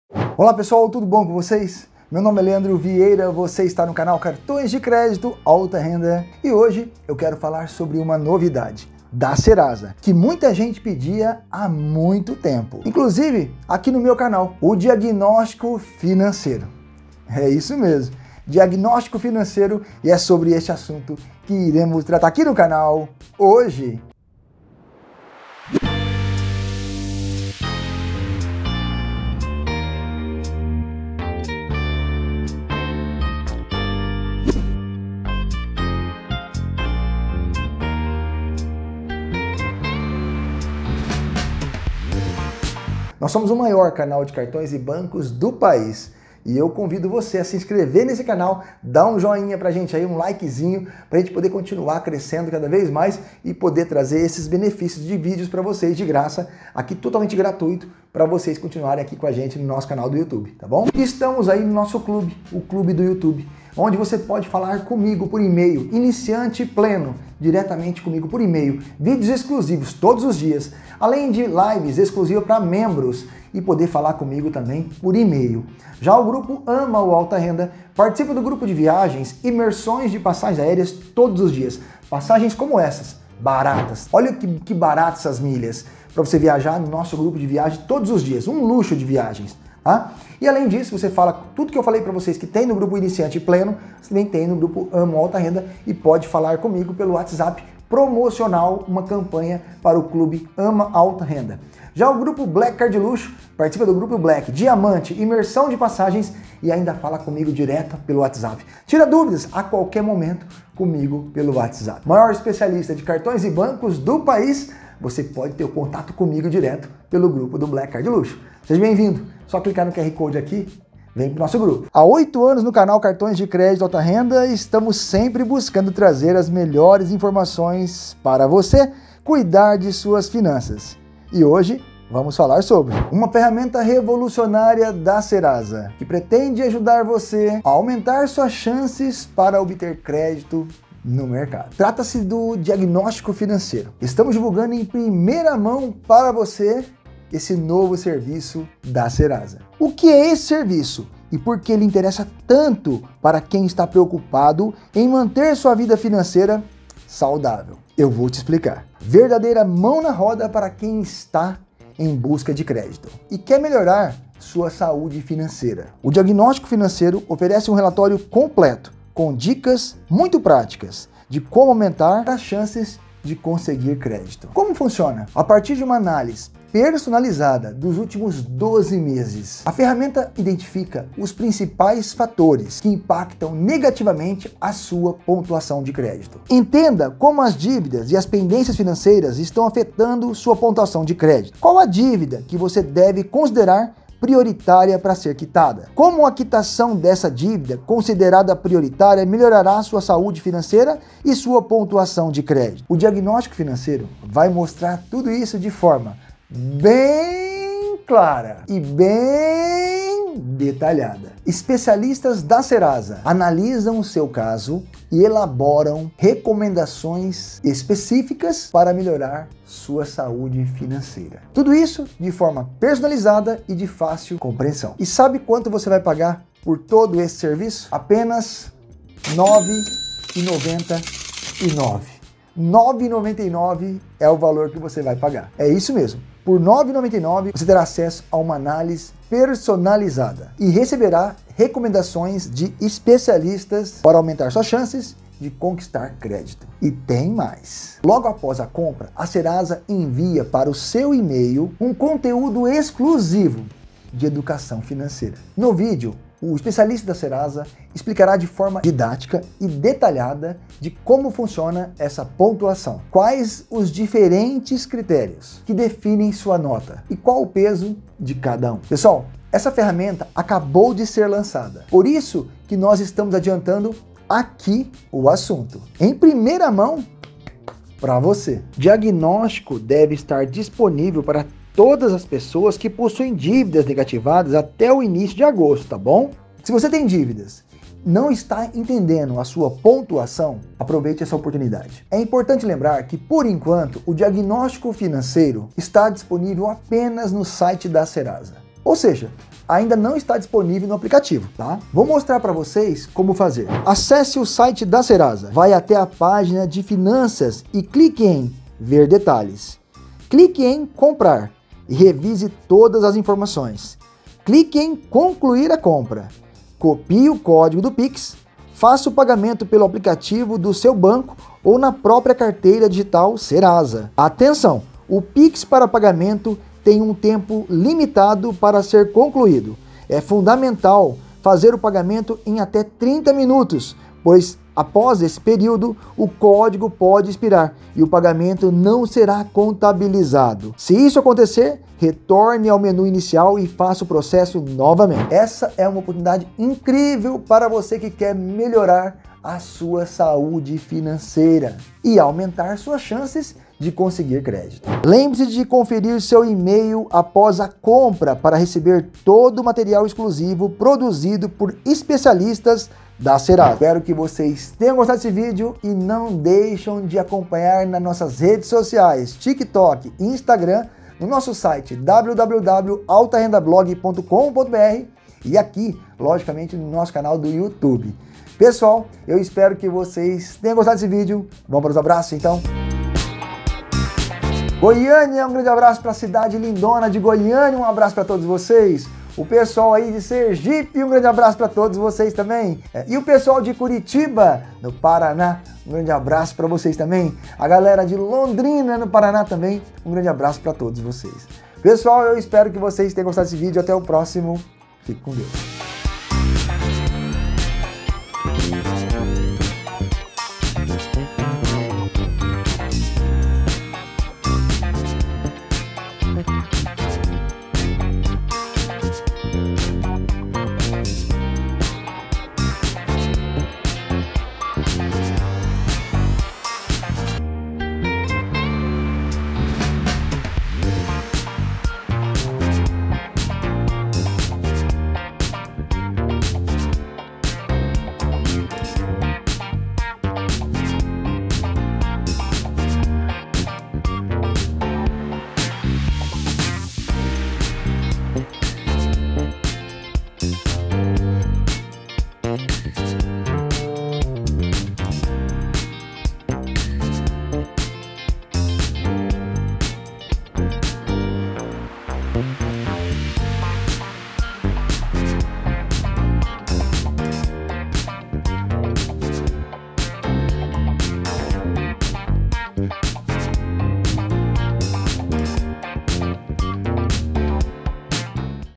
💳GRANDE NOVIDADE! CHEGOU O DIAGNÓSTICO FINANCEIRO DA SERASA, UMA FERRAMENTA PRÁTICA EXCLUSIVA P/VOCÊ
Os brasileiros que estão em busca de crédito contam com um novo serviço da Serasa que auxiliará nas decisões financeiras e guiará o consumidor pelo melhor caminho até a aprovação. Disponível para consumidores negativados, o “Diagnóstico Financeiro” oferece acesso a um relatório financeiro completo com dicas práticas de como aumentar a pontuação de crédito.

A partir de uma análise personalizada do histórico financeiro do  onsumidor nos últimos 12 meses, o serviço identifica os principais fatores que impactam negativamente o Score e oferece recomendações específicas, elaboradas por especialistas da Serasa. O relatório considera os critérios que movimentaram a pontuação ao longo do ano, como o compromisso com crédito, pagamento ou novas dívidas, consultas de empresas aos CPF e a evolução das finanças.

Disponível apenas no site da Serasa, o serviço está disponível por R$ 9,99. Além do relatório, após a compra o consumidor ainda terá acesso a um conteúdo exclusivo sobre saúde financeira por e-mail, que explicará de maneira didática sobre os critérios e pesos que compõem a pontuação.
Importante: A funcionalidade está sendo disponibilizada aos poucos e até o final do mês de julho todos usuários que possuem dívidas estarão aptos a fazer a compra via site da Serasa.

🚀 TicTok  @AltaRendaCartoes